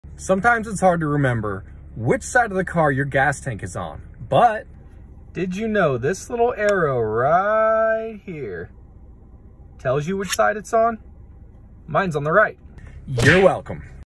How to Tell Which Side of the Car Your Gas Tank is On
This is the easiest way to tell which side of the car your gas tank is on. You're welcome!

If you want to support Steve and The Hart Family, please grab a copy of Steve's Children's Book "Big Plane Small Plane"